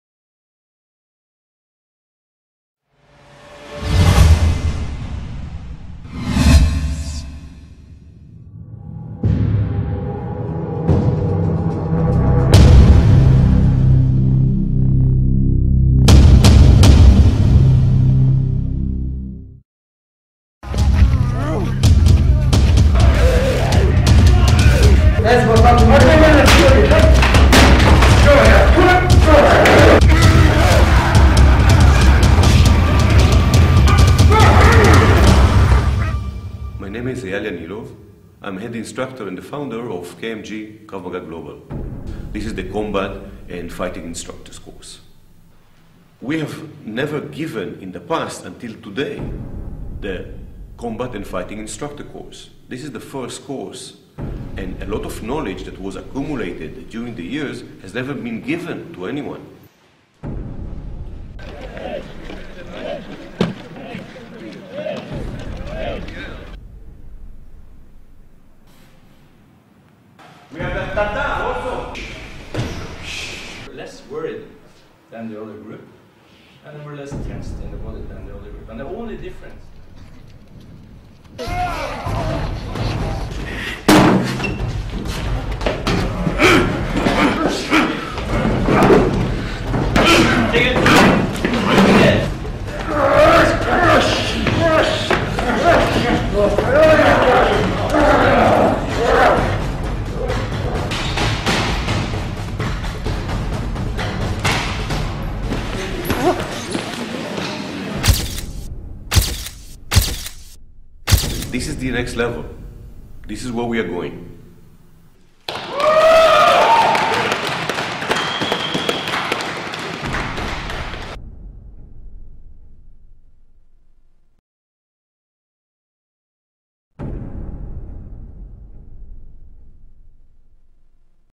Krav Maga Combat and Fighting Instructors Course lead by Eyal Yanilov
Kick Ass Combat & Fighting course
Only the best of the best